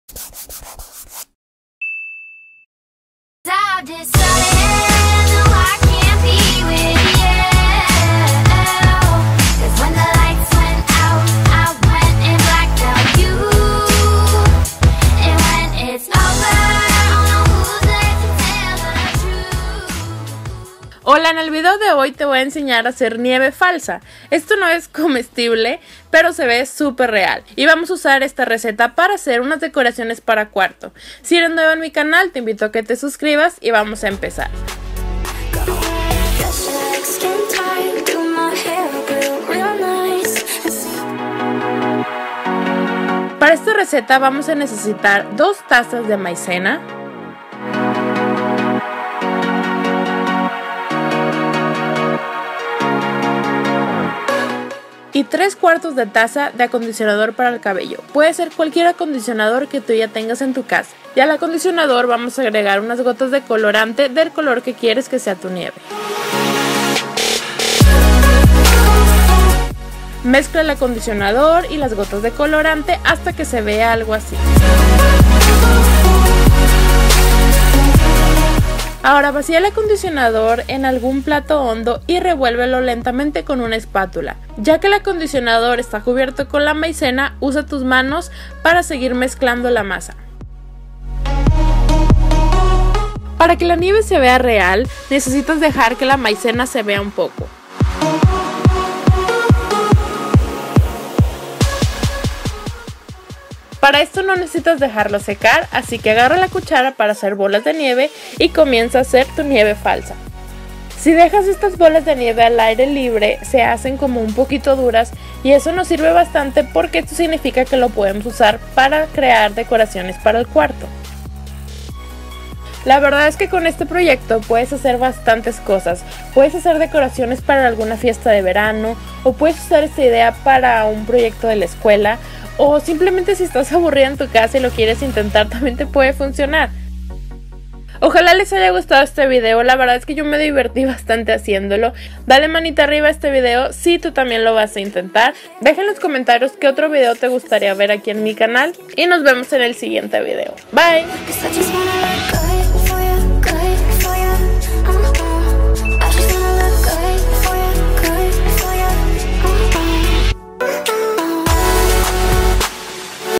COMO HACER HELADO/NIEVE FALSA ♥ DIY
Como hacer nieve falsa que se ve super REAL!
LIKE si te gusto el video!! Ayúdame a llegar a 2,000 likes!! :D

INGREDIENTES:
- 2 Tazas Maizena
- 3/4 Taza Acondicionador para cabello
- Colorante para comida

Si tu mezcla quedo muy aguada agrega mas maizena, y si la sientes muy seca agrega mas acondicionador, la textura debe sentirse como play-dough o plastilina.

Usa hellomaphie en instagram y twitter para que pueda ver las fotos de tus recreaciones :)